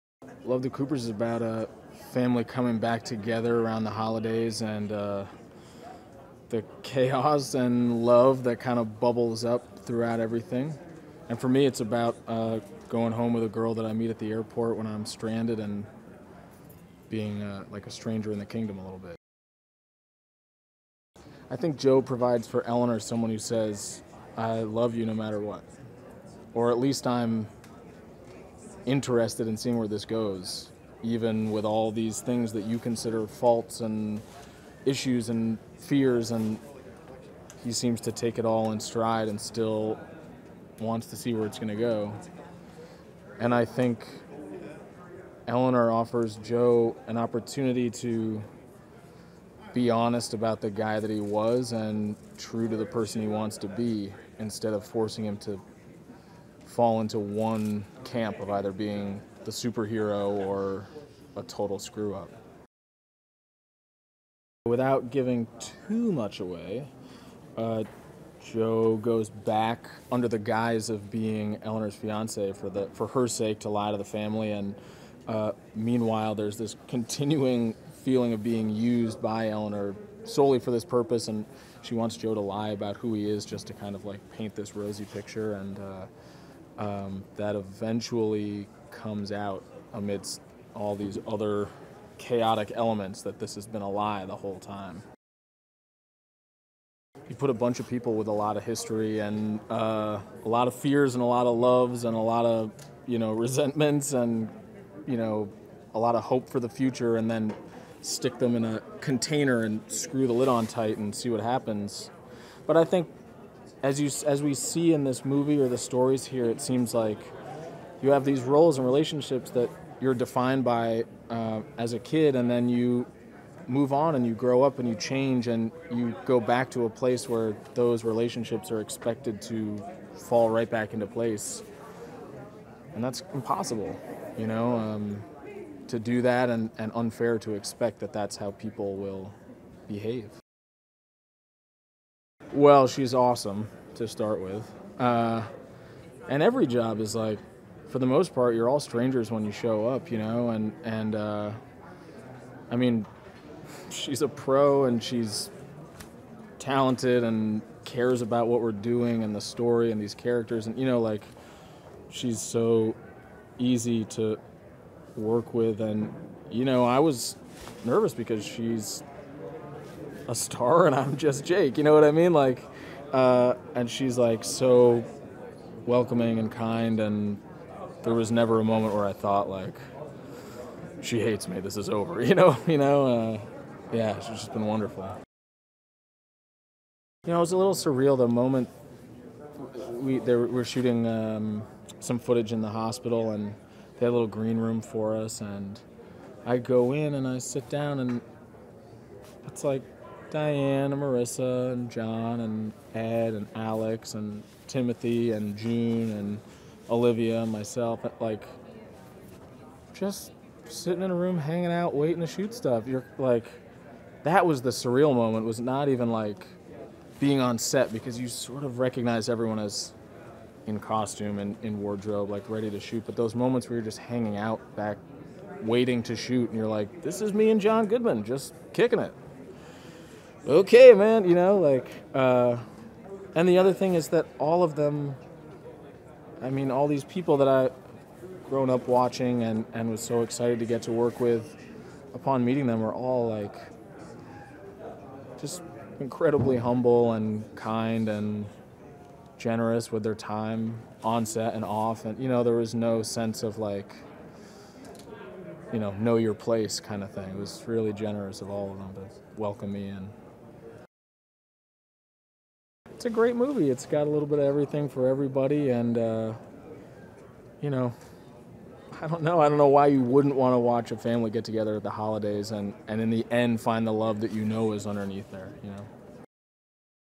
Love The Coopers: Jake Lacy "Joe" Behind-the-Scenes Interview | ScreenSlam
DIRECTOR: Jessie Nelson

CAST: John Goodman, Diane Keaton, Amanda Seyfried, Ed Helms, Alan Arkin, Olivia Wilde, Marisa Tomei, June Squibb, Jake Lacy, Anthony Mackie, Alex Borstein

PLOT: When four generations of the Cooper clan come together for their annual Christmas Eve celebration, a series of unexpected visitors and unlikely events turn the night upside down, leading them all toward a surprising rediscovery of family bonds and the spirit of the holiday.